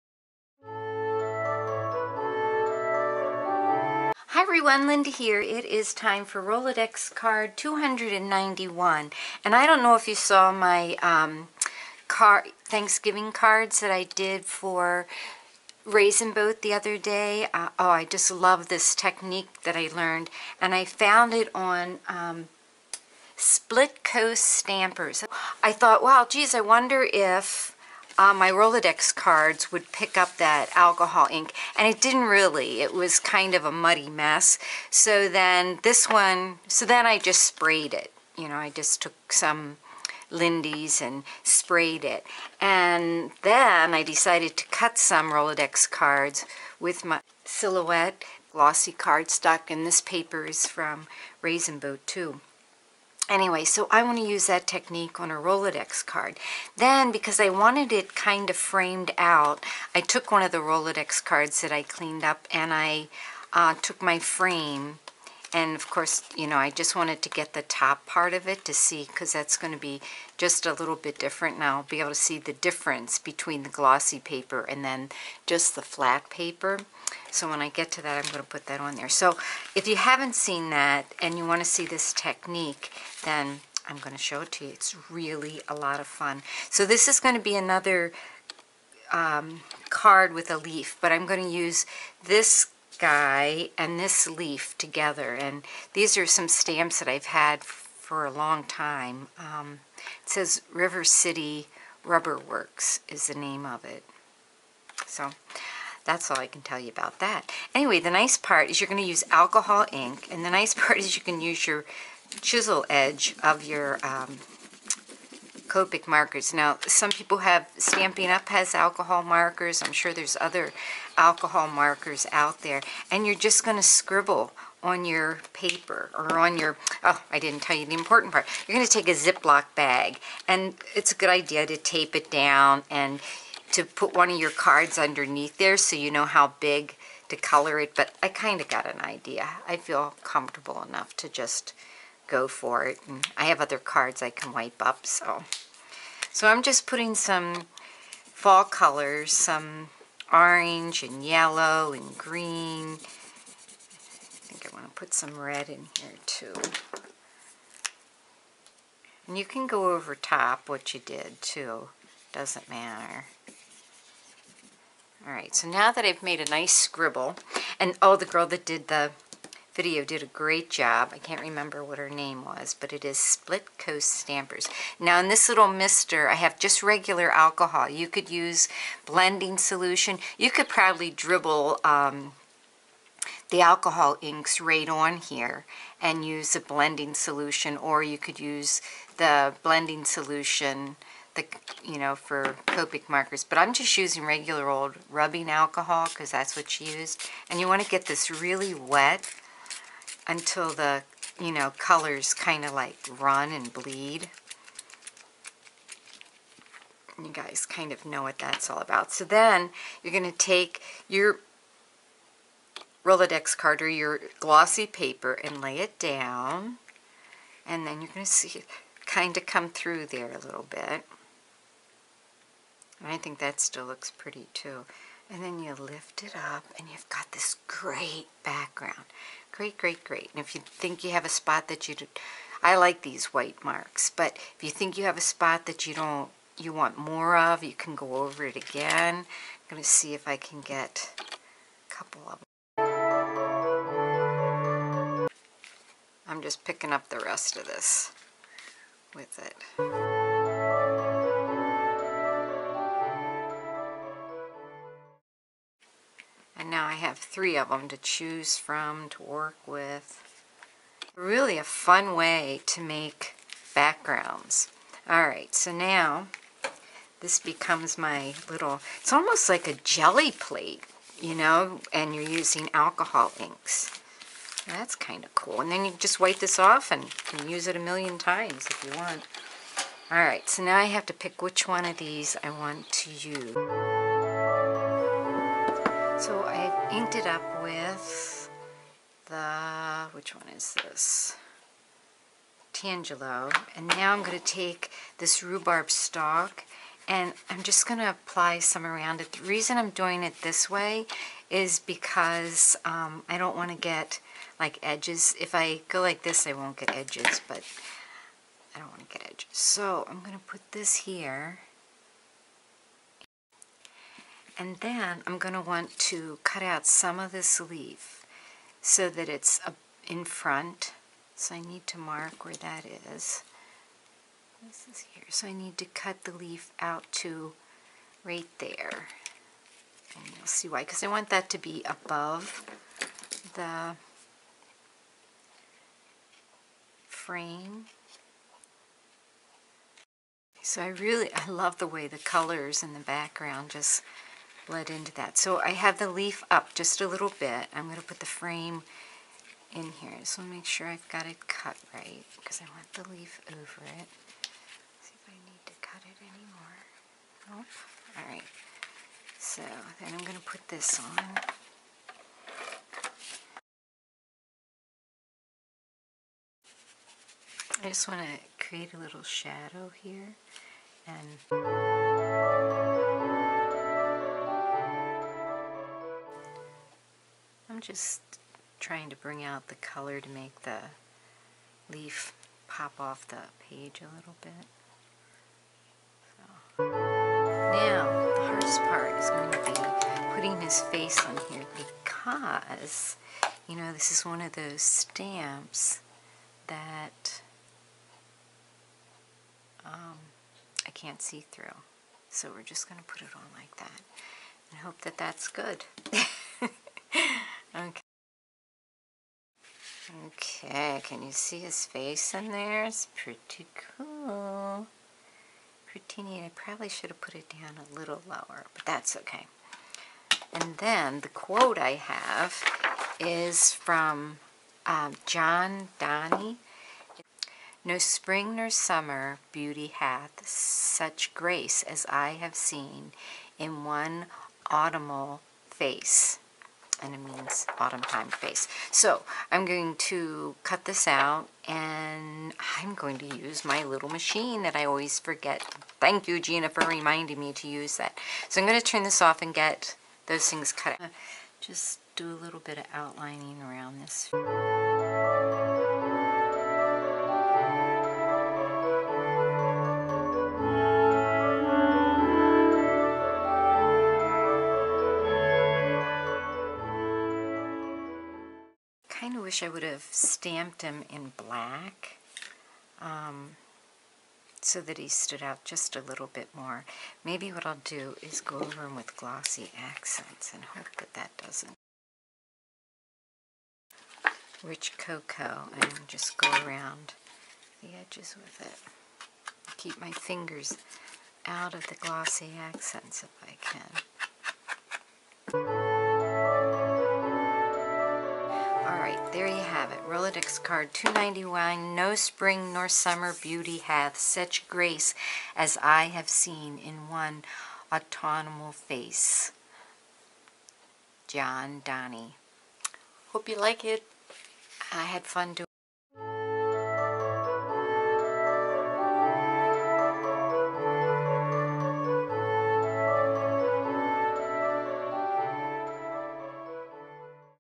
RCAD 291
Rolodex Card A Day 291 was created with the same background technique that I used on the Raisin Boat Challenge card the other day. The quote says, "No spring nor summer beauty hath such grace as I have seen in on autumnal face." ~John Donne~ I had fun with a face and a leaf stamp on this card. I added glossy accents to his eyes and mustache to make them pop.